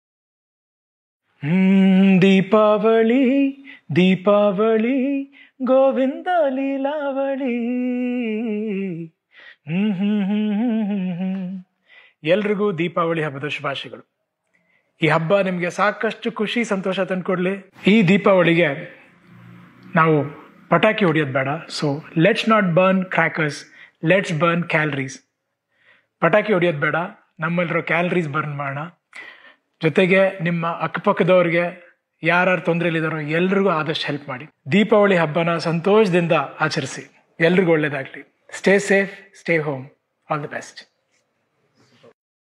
ಹೊಸ ಚಾಲೆಂಜ್ಗೆ ಚಾಲನೆ ನೀಡಿದ ಅಪ್ಪು |lets not burn crackers instead of Burn Calories
In-depth Coverage of Karnataka’s Politics |  Regional News  | The Widest Cinema Coverage | Interesting Stories from Across the karnataka.